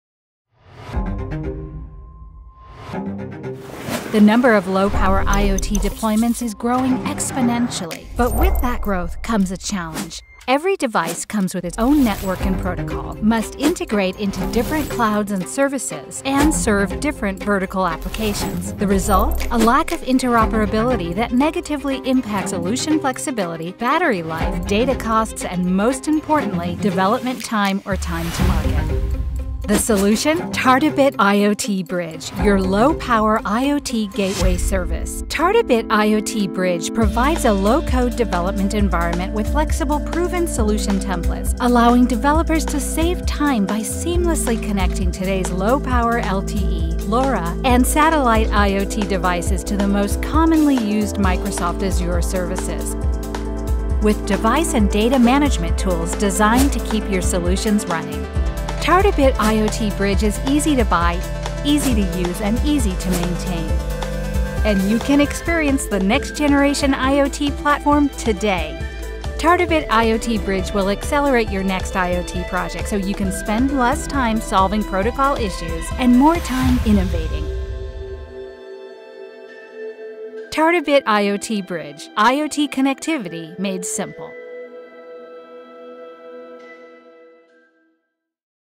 Tartabit IoT Bridge Introduction
We have been asked many times to make it easier to understand the IoT Bridge and that's exactly what we've prepared for you in this 1 minute and 25 second video. Take a look and see how we did.

Tartabit IoT Bridge makes integration between LPWAN (LORA, NBIOT, SAT, CAT-M) devices and industry leading cloud services easier than ever.